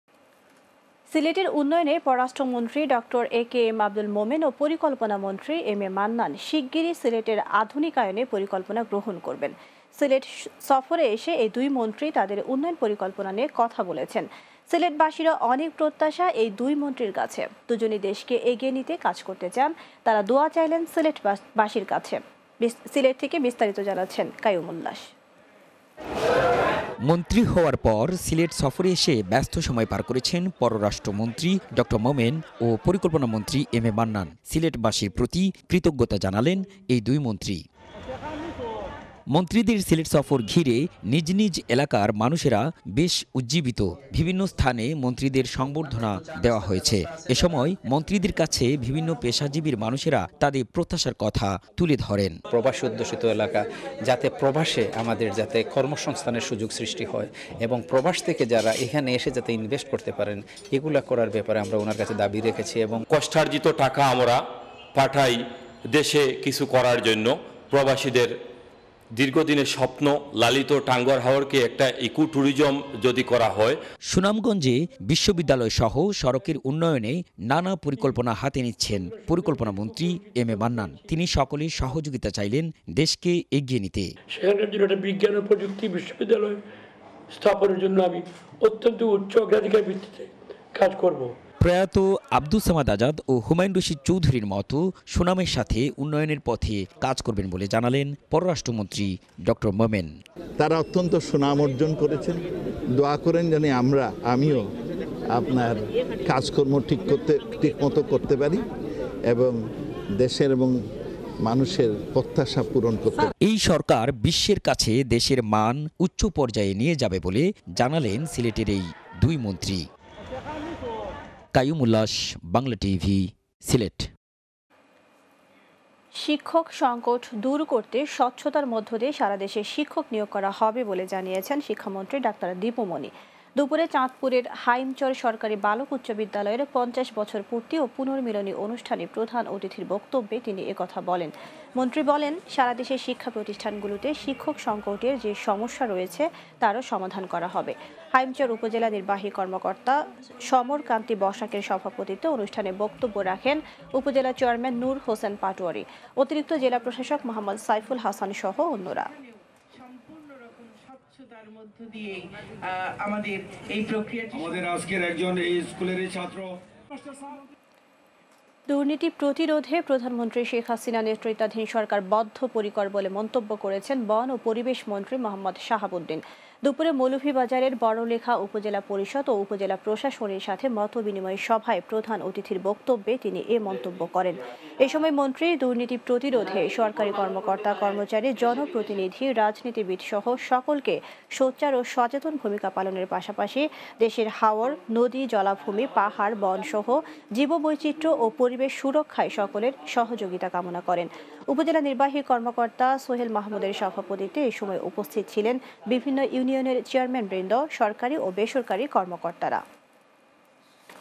সিলেটের আধুনিকায়নে পরিকল্পনা গ্রহণ করবেন পরিকল্পনা মন্ত্রী
সিলেটের উন্নয়নে পররাষ্ট্রমন্ত্রী ড. এ কে আব্দুল মোমেন ও পরিকল্পনা মন্ত্রী এম এ মান্নান শিগগিরই সিলেটের আধুনিকায়নে পরিকল্পনা গ্রহণ করবেন। সিলেট সফরে এসে এই দুই মন্ত্রী তাদের উন্নয়ন পরিকল্পনা নিয়ে কথা বলেছেন। সিলেটবাসীরও অনেক প্রত্যাশা এই দুই মন্ত্রীর কাছে। দুজনই দেশকে এগিয়ে নিতে কাজ করতে চান। তারা দোয়া চাইলেন সিলেটবাসীর কাছে। সিলেট থেকে বিস্তারিত জানাচ্ছেন কাইয়ুম উল্লাস।